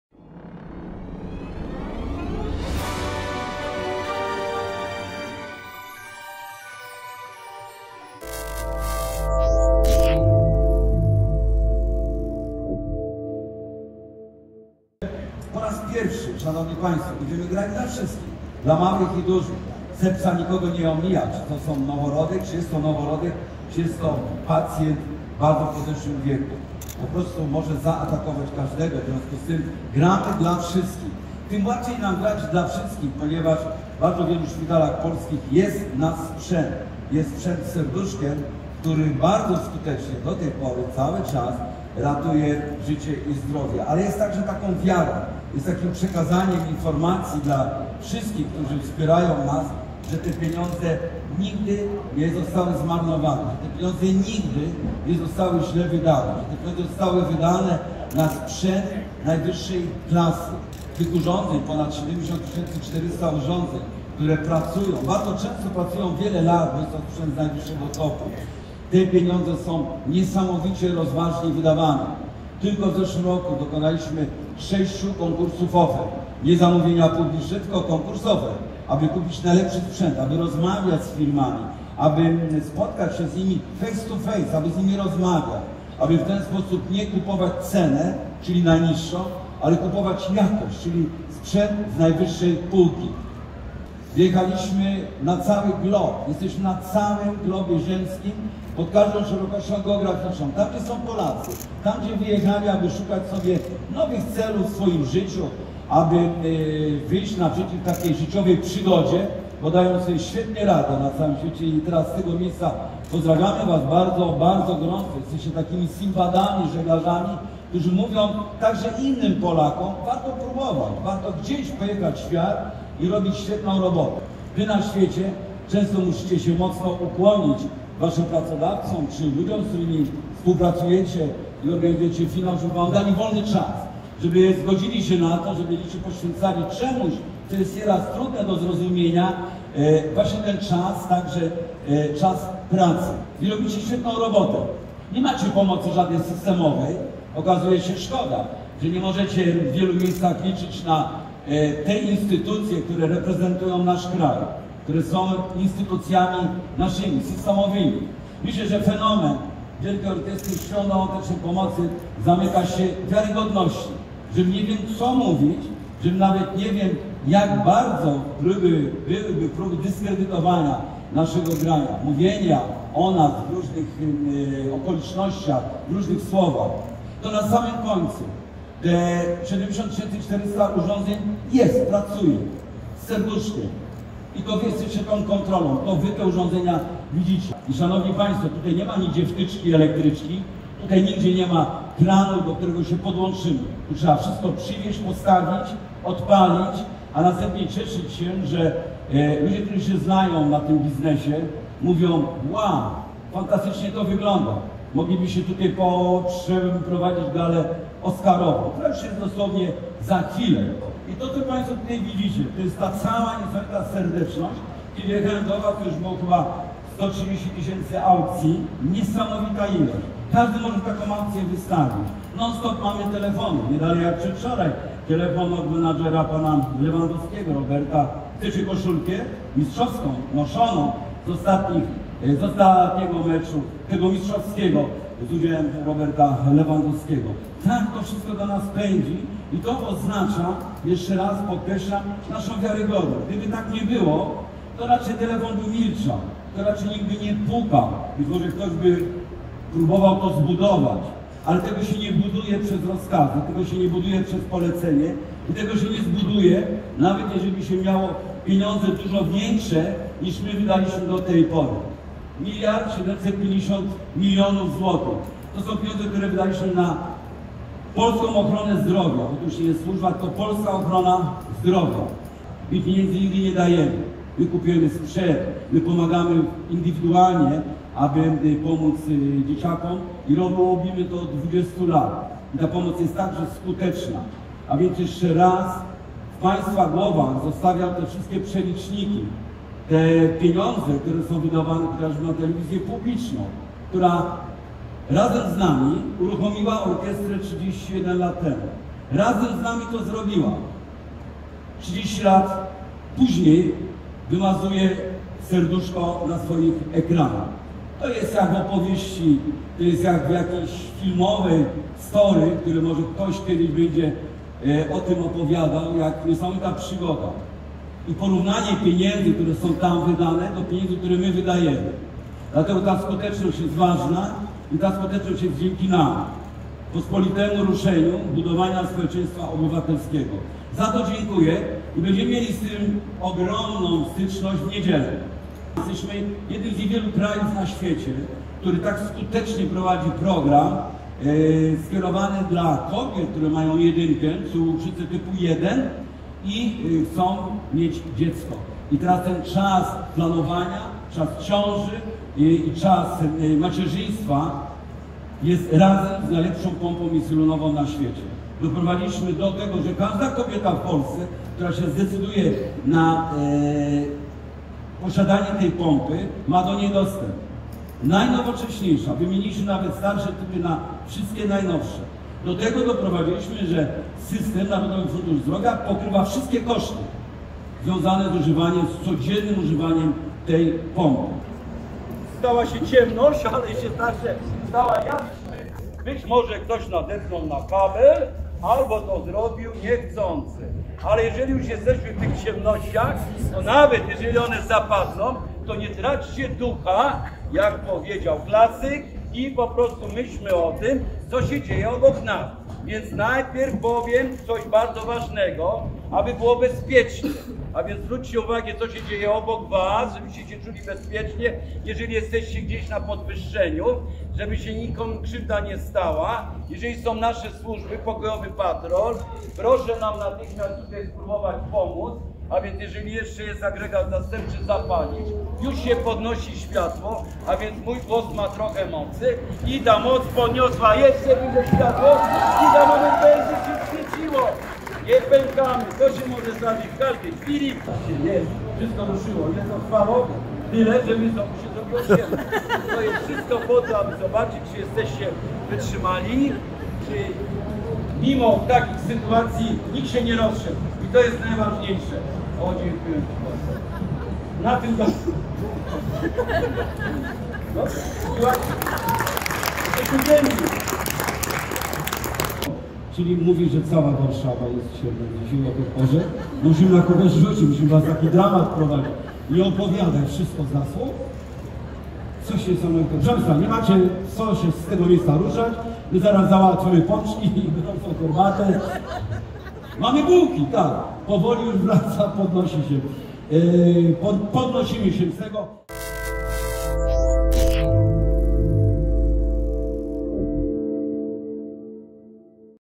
31. Finał WOŚP – zapowiedź. Fundacja Wielka Orkiestra Świątecznej Pomocy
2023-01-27
Fundacja Wielka Orkiestra Świątecznej Pomocy to ciesząca się największym zaufaniem społecznym organizacja w Polsce. Założona została w 1993 roku i od tego czasu co roku, w styczniu, organizuje zbiórkę pieniędzy, która nazywa się Finałem WOŚP. Za zebrane środki Fundacja WOŚP kupuje i przekazuje w darze szpitalom dziecięcym w Polsce najnowocześniejszy sprzęt medyczny. Od początku swojej działalności Fundacja zebrała na wsparcie polskiej medycyny sumę ponad 1,75 mld PLN i kupiła blisko 70 tys. urządzeń. 31. Finał WOŚP odbędzie się 29 stycznia 2023 roku. Celem zbiórki jest walka z sepsą. Finał dla wszystkich - małych i dużych!